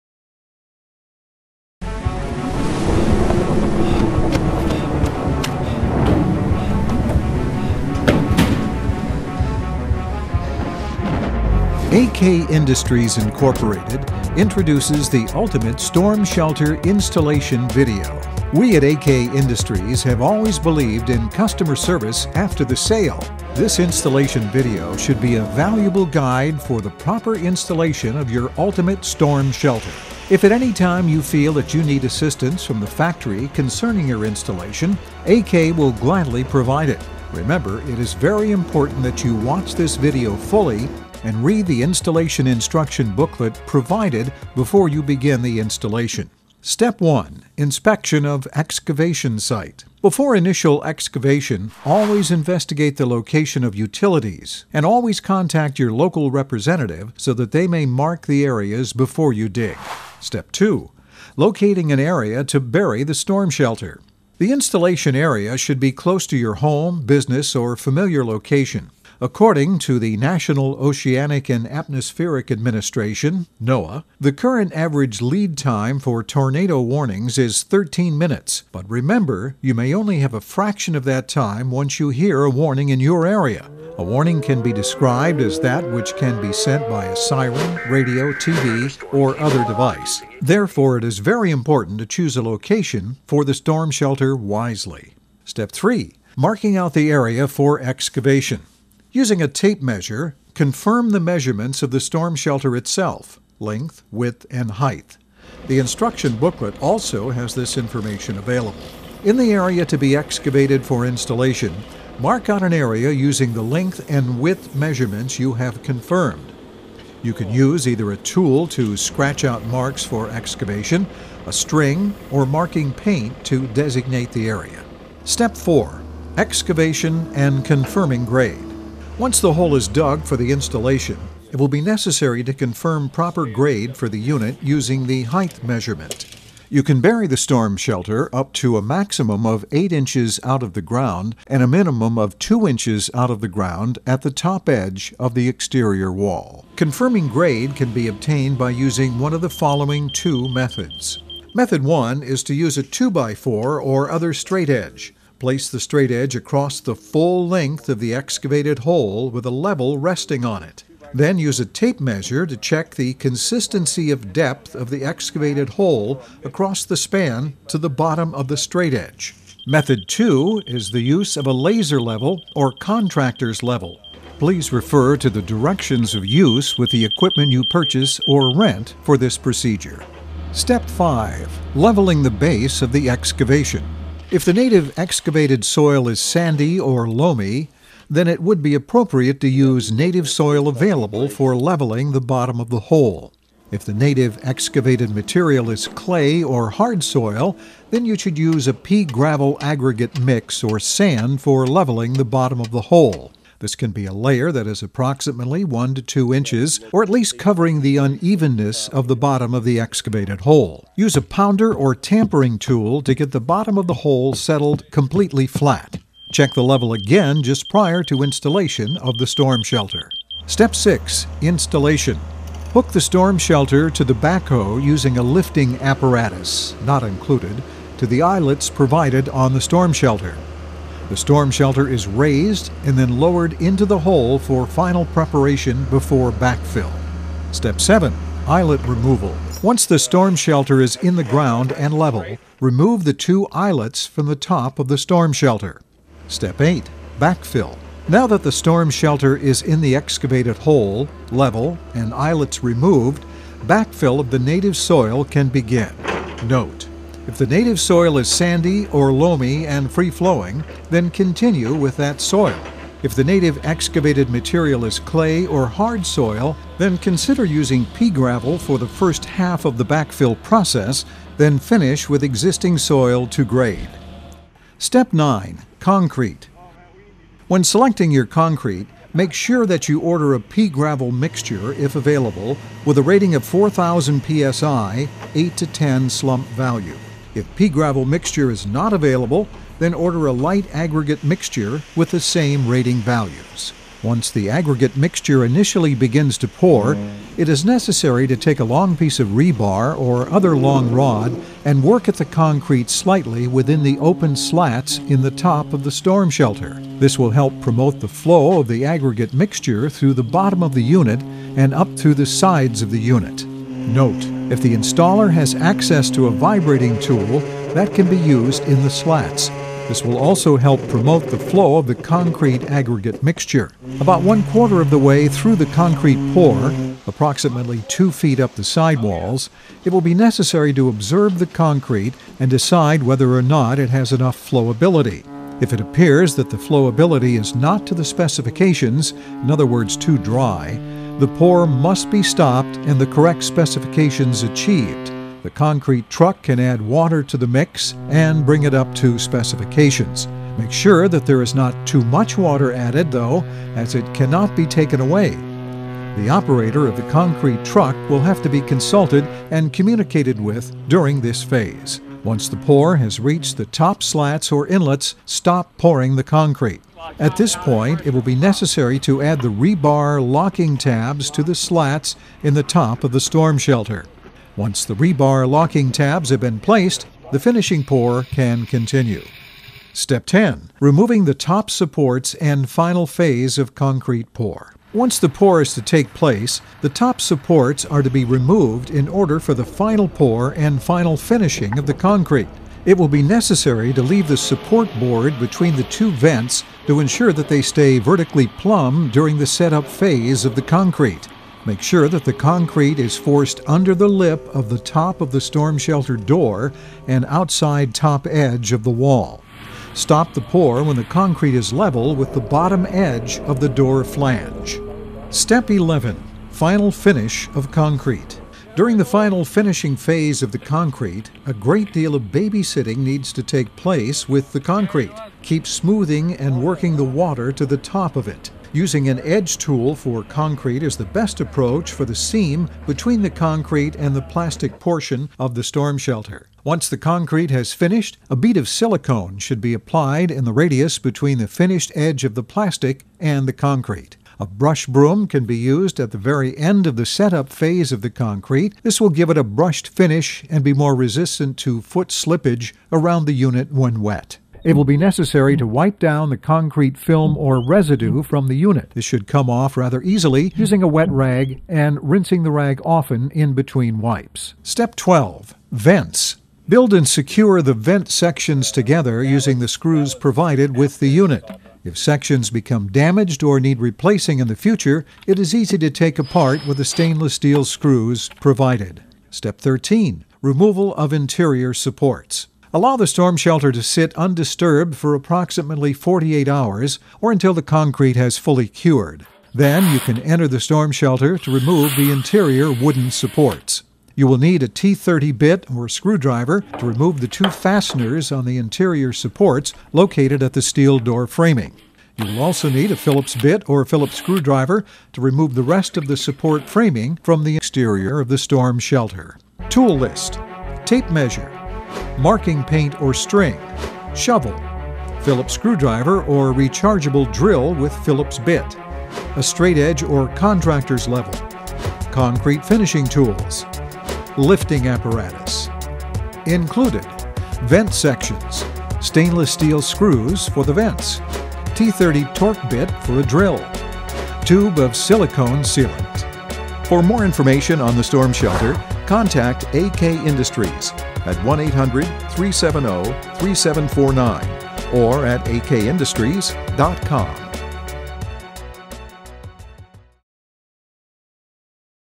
AK INDUSTRIES ULTIMATE STORM SHELTER INSTALLATION INSTRUCTION VIDEO
AK Industries, Inc. designed this storm shelter with two purposes in mind: providing affordable protection from violent storms and producing very strong storm shelters in a more cost-effective solution than conventionally installed shelters. This storm shelter design functions as a mold for concrete filling between the ¼" (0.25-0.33 inch) inner and outer walls that act as a mold form for the concrete fill. The strength of the shelter comes from engineered ribbing and from internal structural cores, which reinforce the inner and outer walls. It is also built strong enough to be buried without concrete filling in between walls. Concrete is only necessary for the lid piece due to traffic load ratings. This provides installers the two options that are both safe and reliable. The shelter provides 158 cubic feet of total internal space with seating for up to twelve people with standing room as well.